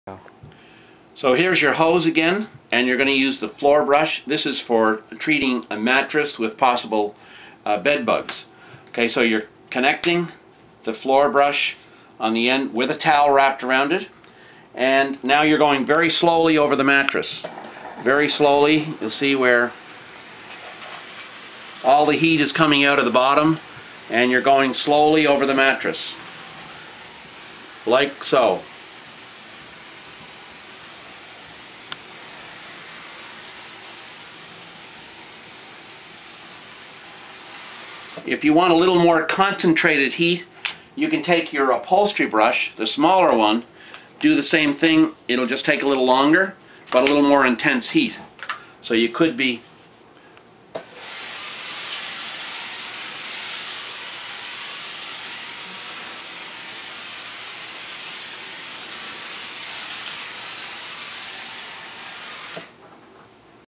Clean Greener -Sanatize Mattress (bed bugs)
showing how to hold and move brush while getting rid of bed bugs